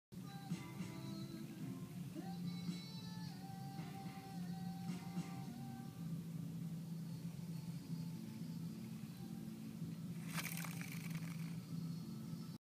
Cardboard gears | Stubby teeth
Very simple gear geometry. 24 and 12 teeth.